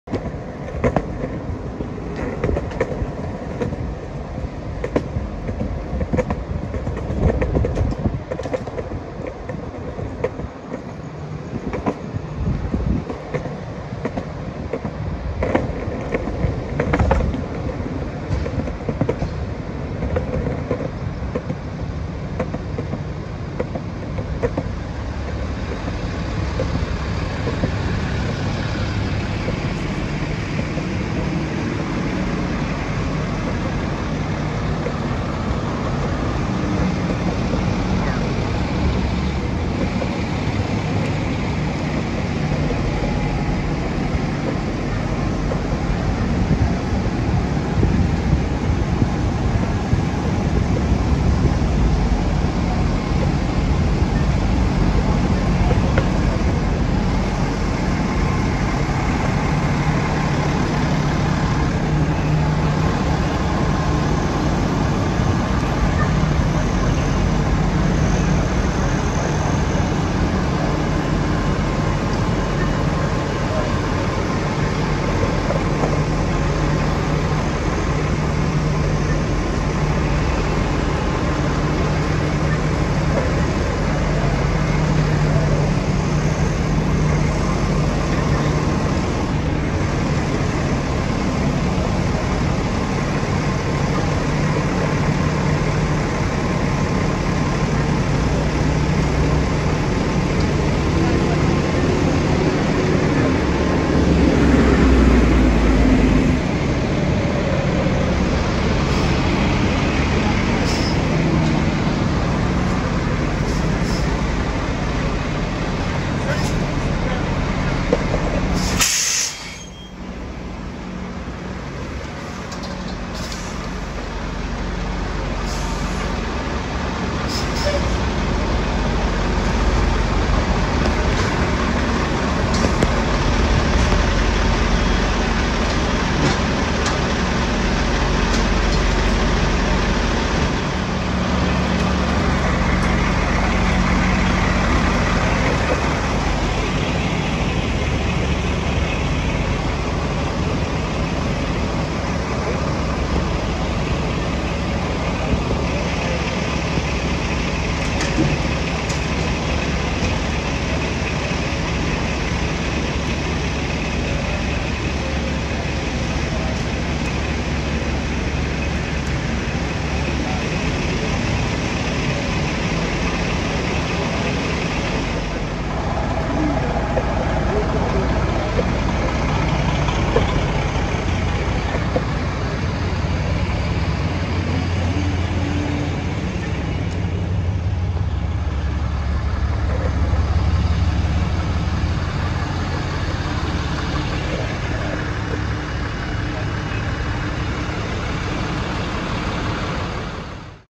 LAFD Extinguish Fire At Hamptons818 Restaurant In Sherman Oaks
LAFD on the scene of a restaurant fire at the Hamptons818 On Ventura Blvd in Sherman Oaks, California during the quarantine.